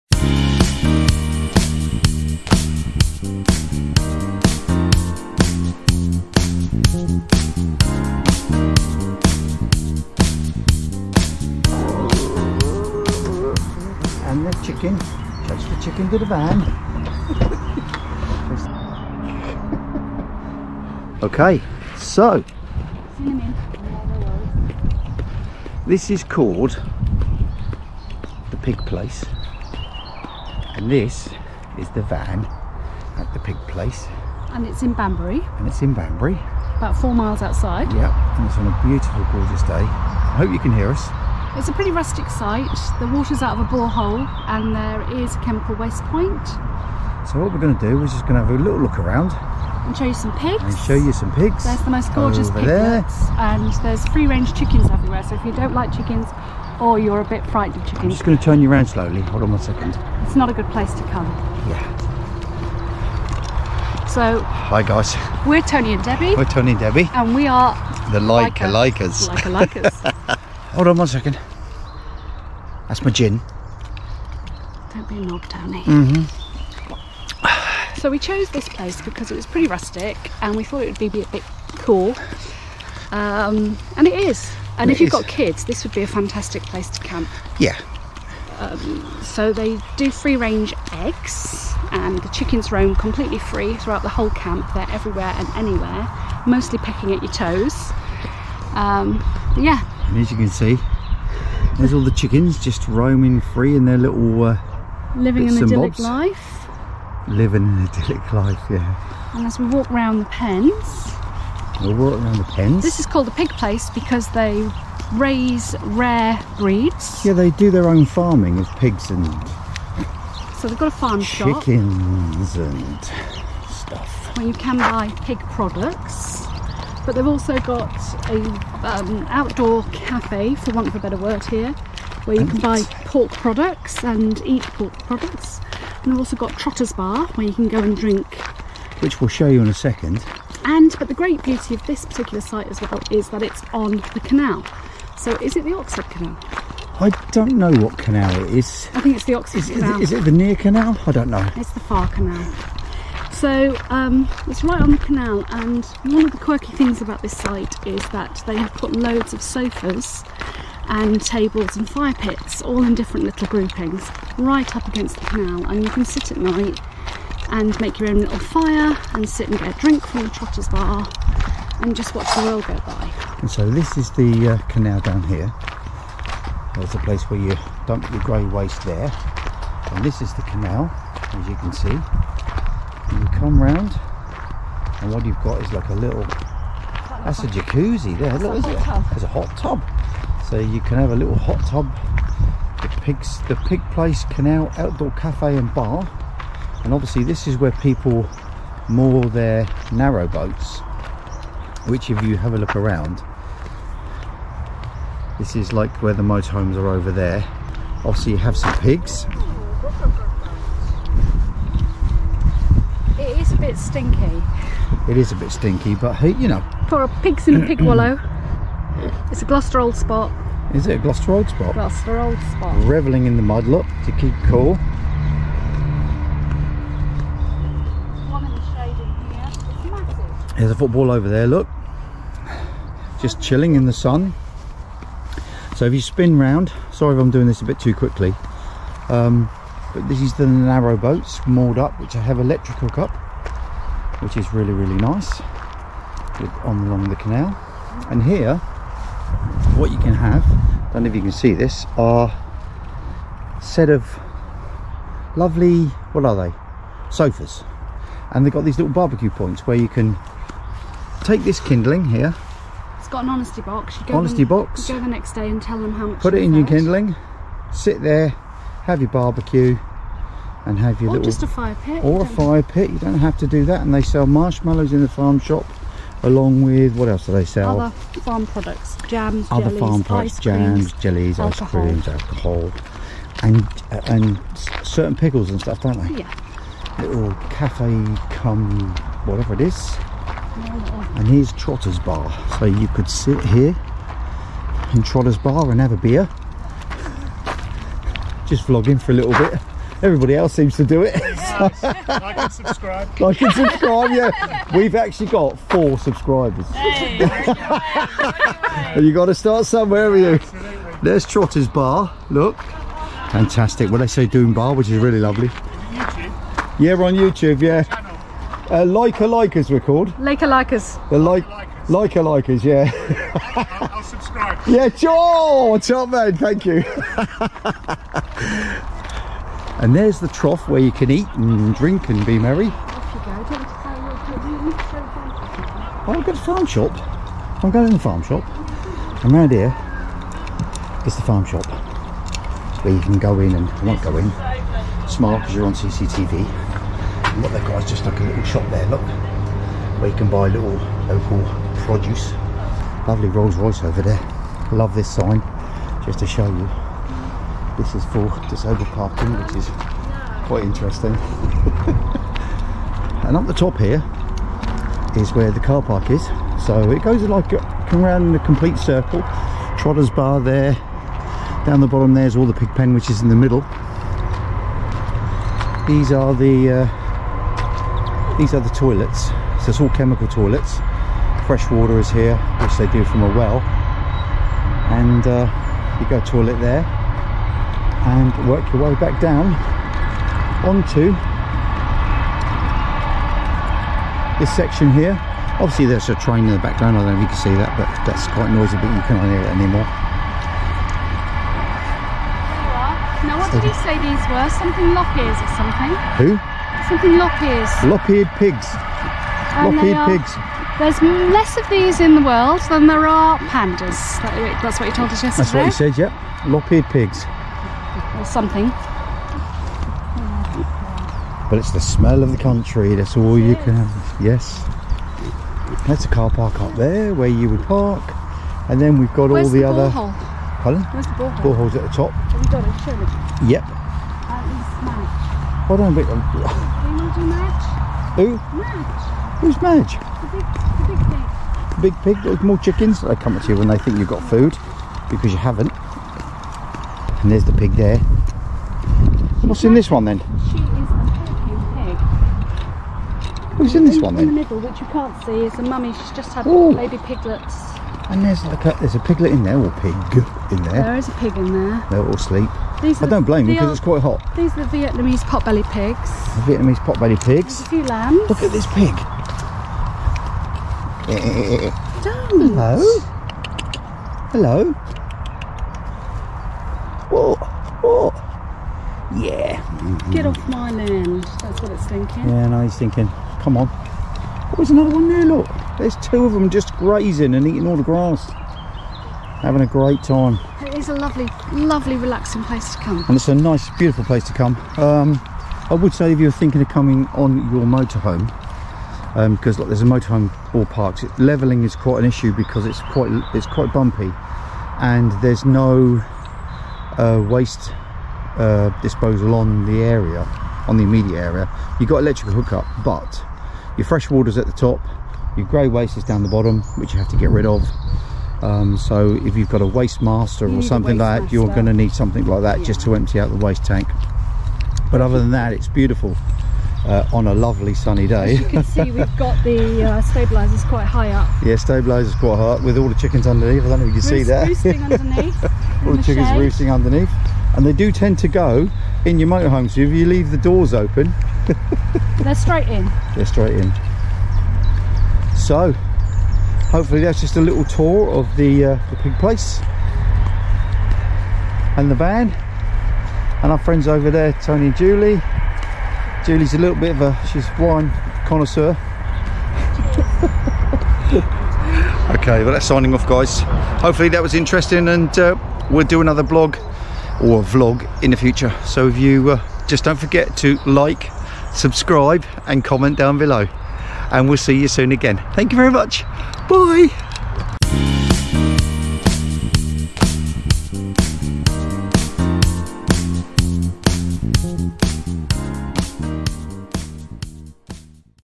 Motorhome Camping at THE PIG PLACE, Banbury - a chicken stole my steak
Camping at THE PIG PLACE in our LAIKA KREOS 5009 motorhome.
Take a look around The Pig Place - such a cool place to stop for a few days.
No sooner had we filmed the VLOG when we set up the CADAC and popped on some steaks. Can you belive a chicken pecked my fillet steak off my plate and ran off with it. It was soon joined by 50 of its friends and it was the funniest thing we have seen in such a long time! I didn't get my steak back, but then, not sure I wanted it after they had all had a good peck at it.

Sit by the Oxford canal on old but comfy sofas. Light a fire pit, drink at trotters bar and eat at the Trough. A lovely, laid back, relaxed couple of days at THE PIG PLACE, near Banbury.